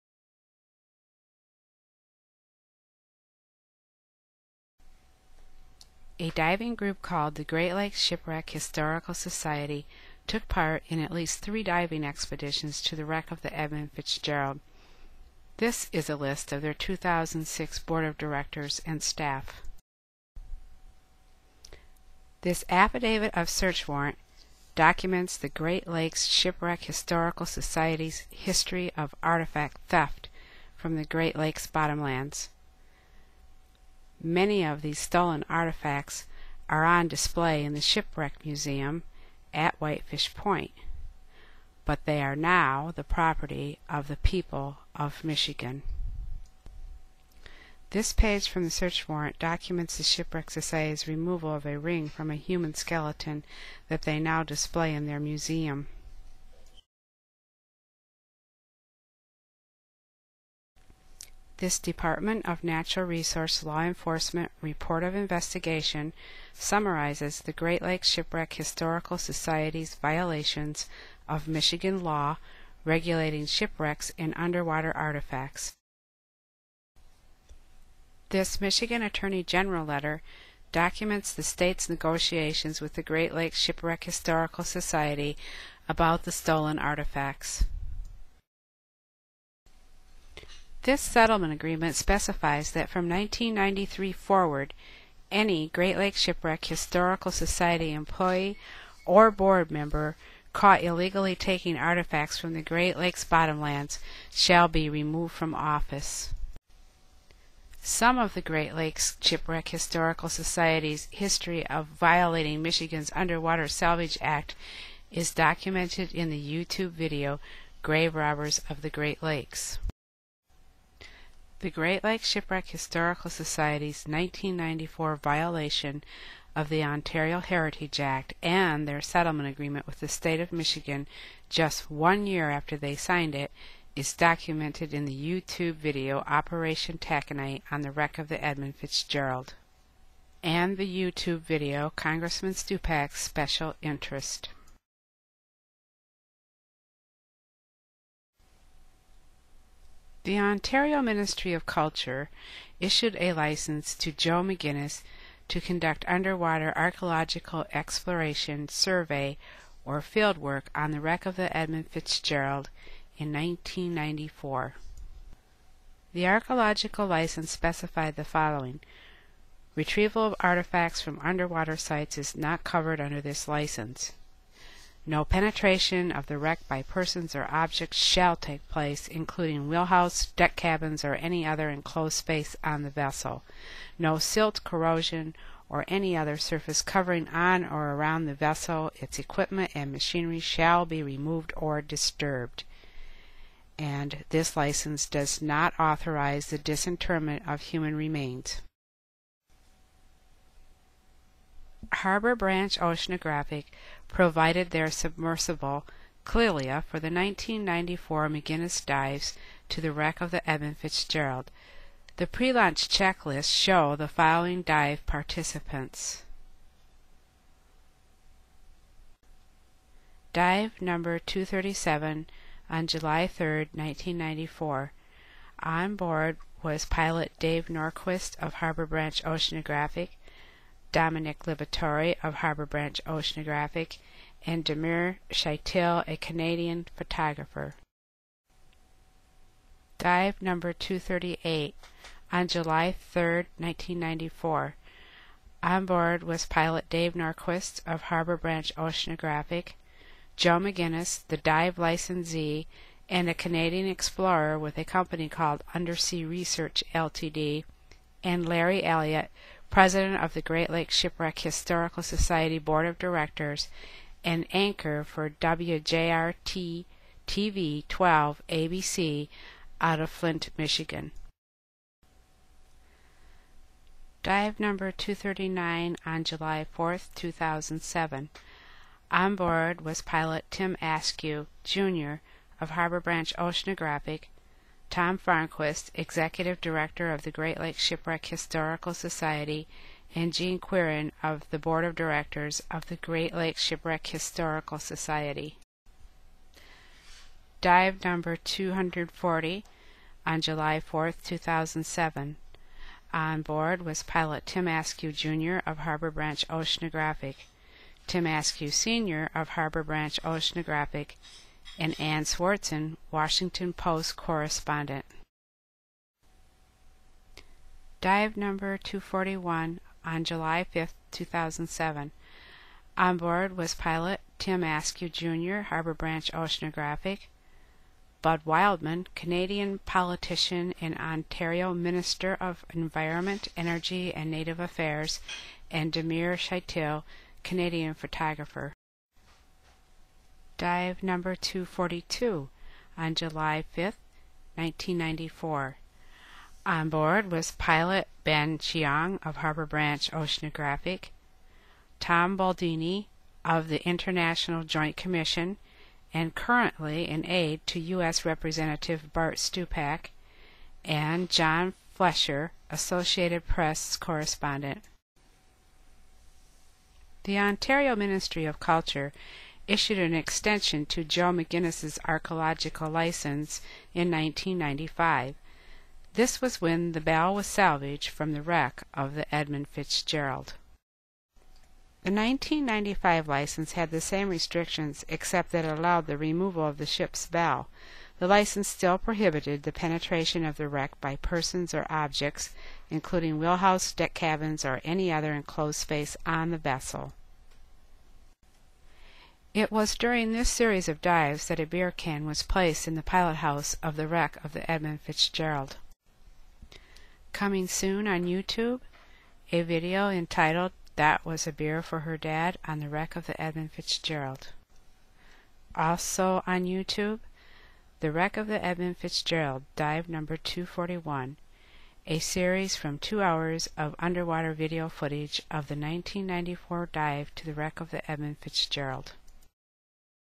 The Wreck of the Edmund Fitzgerald: Historical Documents
This video includes historical documents and underwater video footage of the wreck of the Edmund Fitzgerald that has never been released  to the general public.